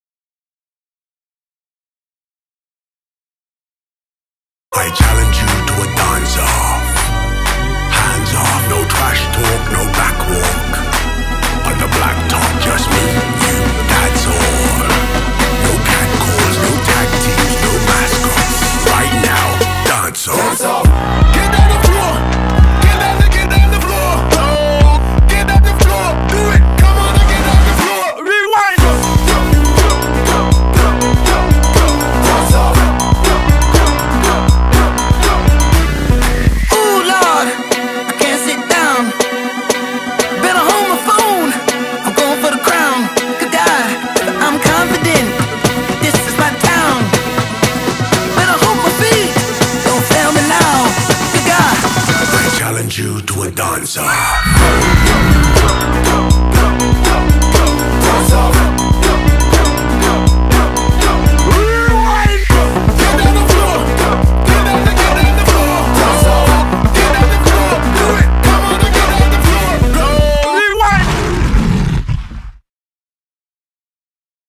Knightro's MascotDanceOffChallenge
Macklemore may have said "no mascots," but Knightro didn't hear him.